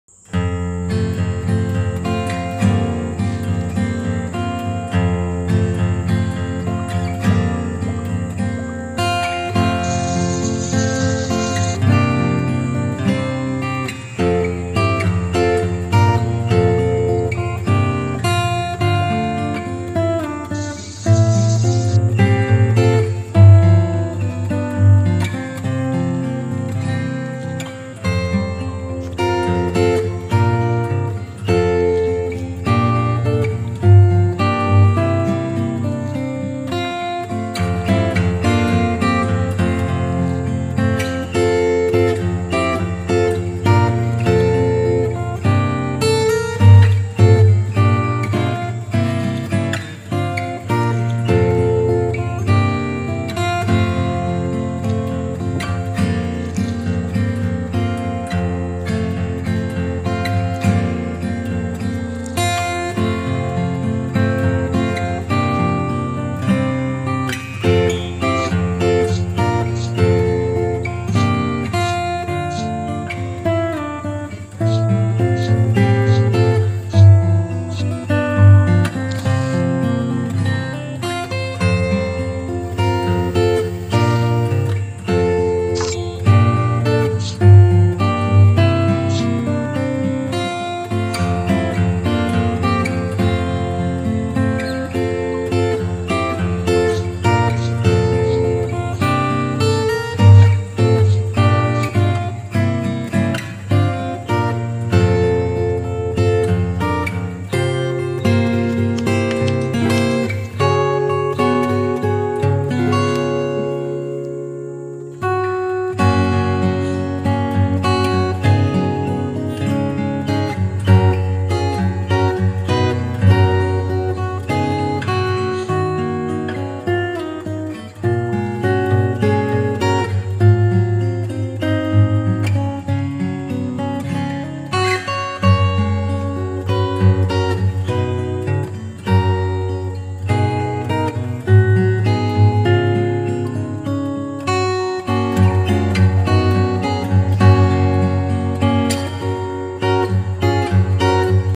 ASMR || Makeup Transformation || Black Beauty Make up
this animation video contains whitehead and blackhead extractions, pimple popping also makeup application.
Makeup ASMR Transformation| Homeless asmr stop motion animation| asmr makeup